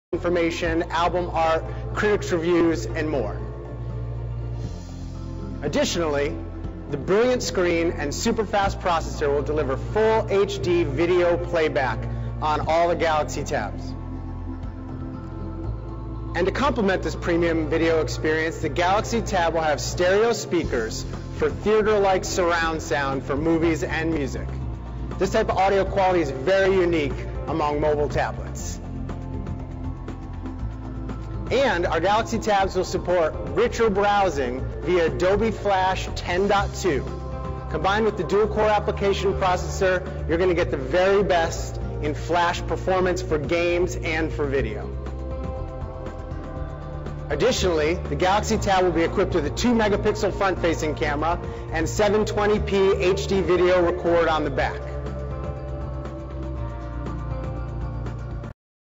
Samsung Media Hub and Features of the new Galaxy Tab 8.9 and 10.1
http // - The new Samsung media hub and the features for the new Samsung Galaxy Tab 8.9 and 10.1